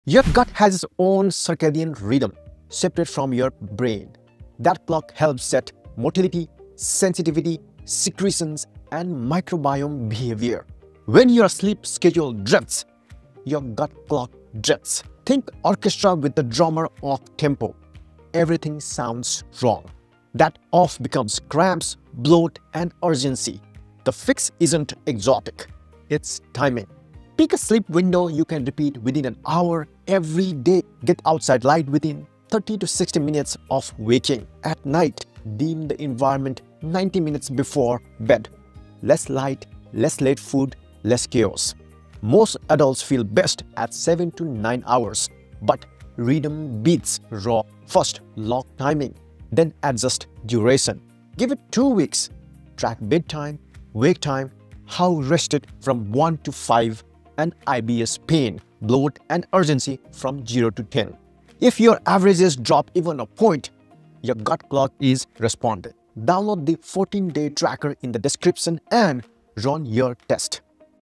Your Gut Has Its Own Clock (And It Might Be Offbeat)
If your cramps, bloating, or urgency feel unpredictable, your gut’s internal clock may be out of sync. The fix isn’t fancy — it’s about timing, consistency, and a simple 2-week experiment you can run yourself.
Watch, then test it.